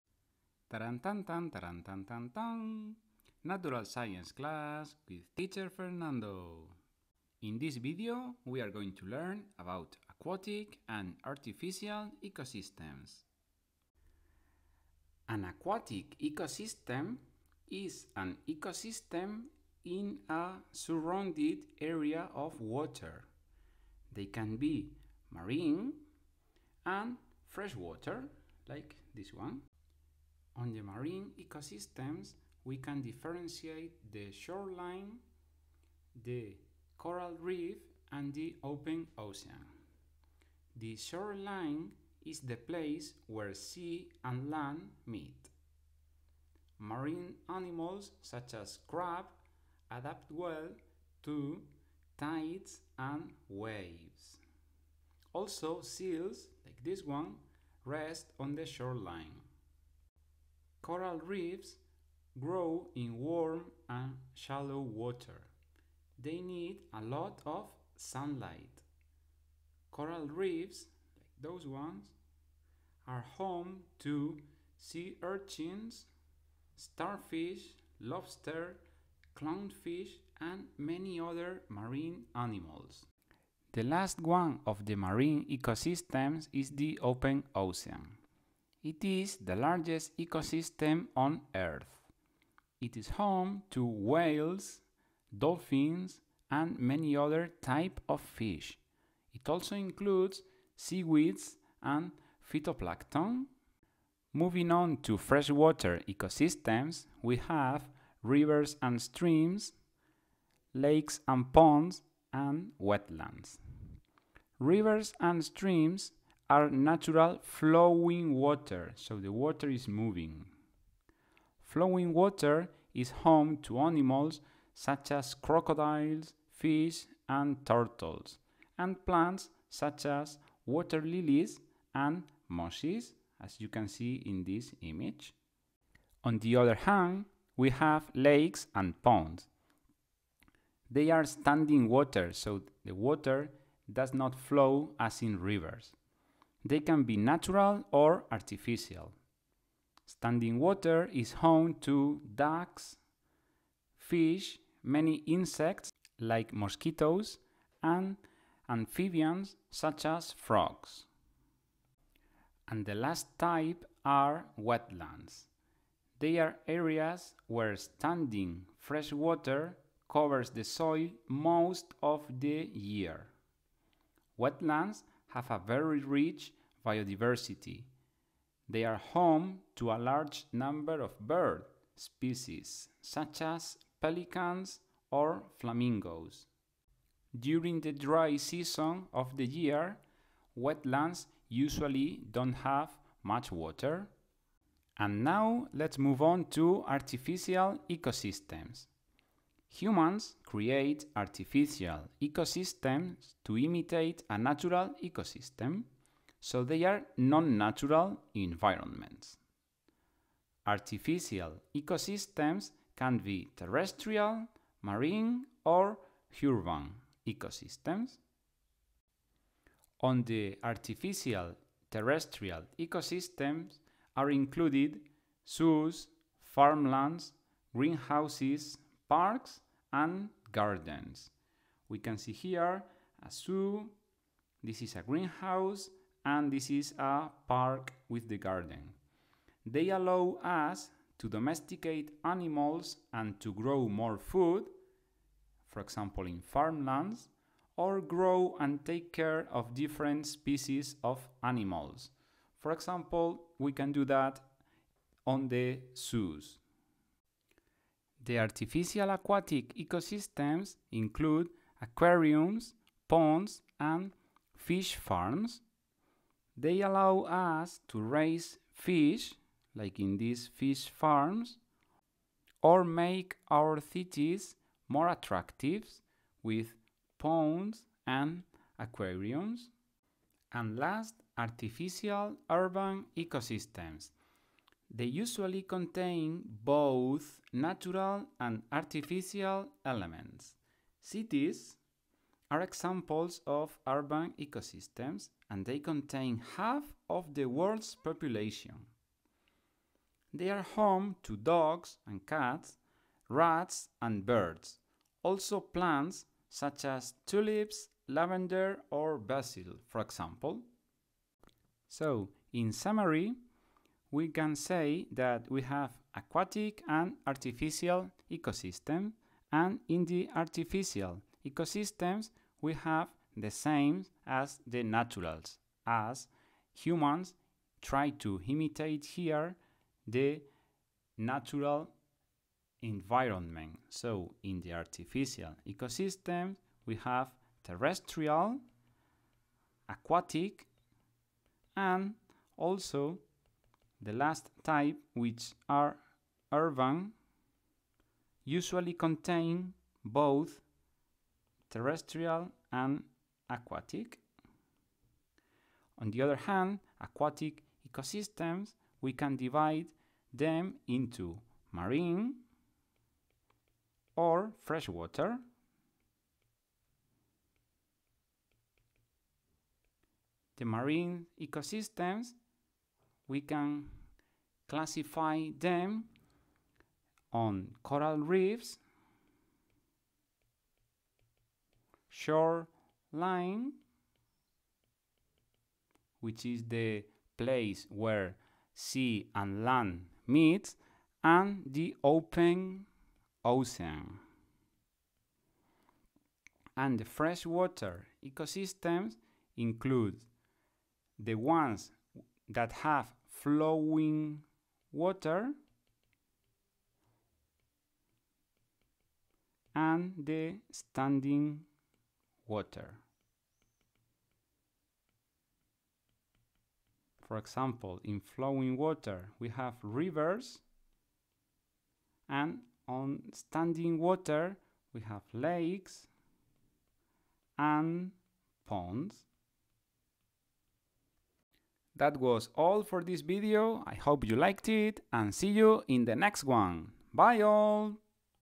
Aquatic and artificial ecosystems
A Natural Science lesson about aquatic and artificial ecosystems.